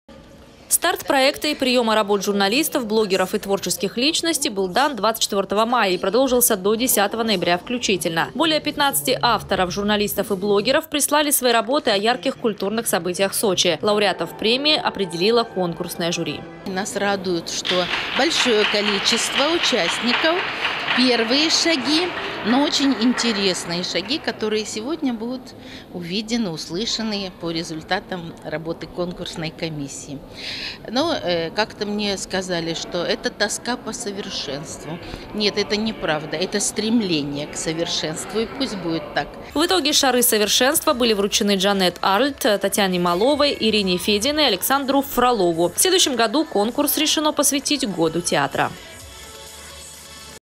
В Сочи вручили «Шар совершенства». Новости Эфкате
Так назвали первый открытый конкурс журналистских и блогерских работ. Его проводит Сочинское концертно-филармоническое объединение.

Старт проекта и приема работ журналистов, блогеров и творческих личностей был дан 24 мая и продолжился до 10 ноября включительно. Более 15 авторов – журналистов и блогеров прислали свои работы о ярких культурных событиях Сочи. Лауреатов премии определило конкурсное жюри.

В итоге “Шары совершенства” были вручены Дженнет Арльт, Татьяне Маловой, Ирине Фединой и Александру Фролову. В следующем году конкурс решено посвятить Году Театра.